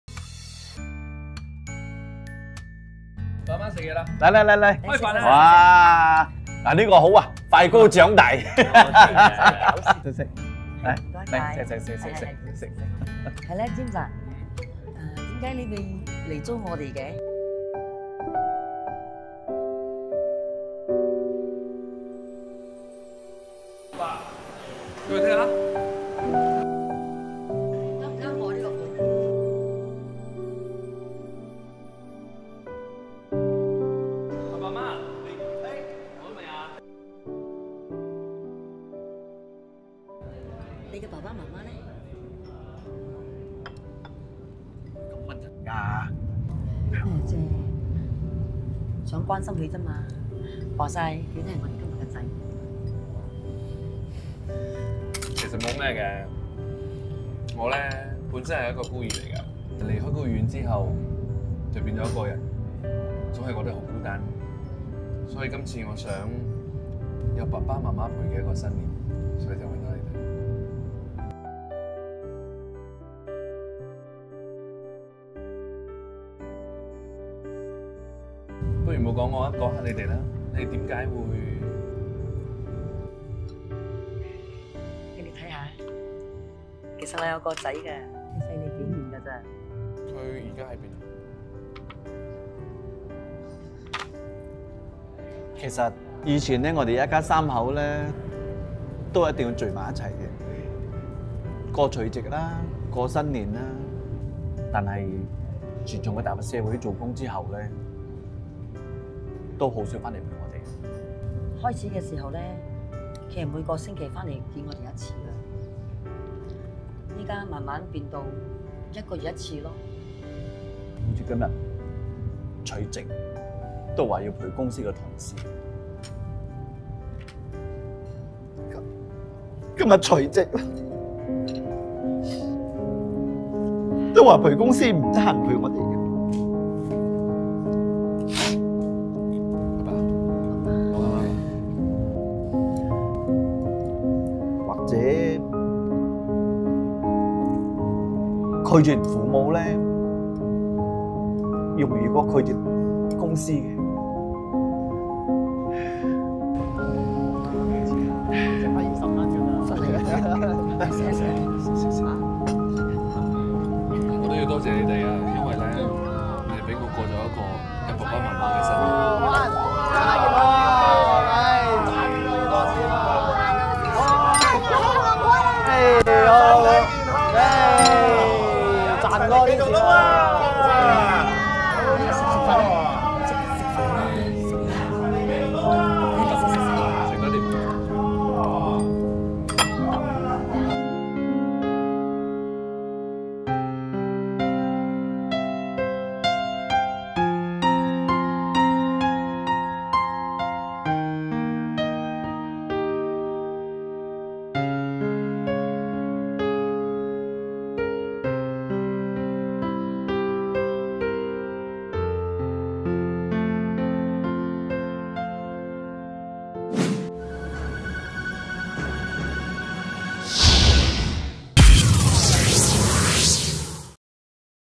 【MY x GSC Movies 新年影片】父母可以等你! 但时间不等父母
有人因为没父母所以渴望有父母的新年
有人却因为“很忙”所以“丢下了”父母 😢

切记，父母可以等你
但时间不等父母. 💔

MY 和 GSCMOVIES 祝你新年快乐，尽早回家吧 ❤️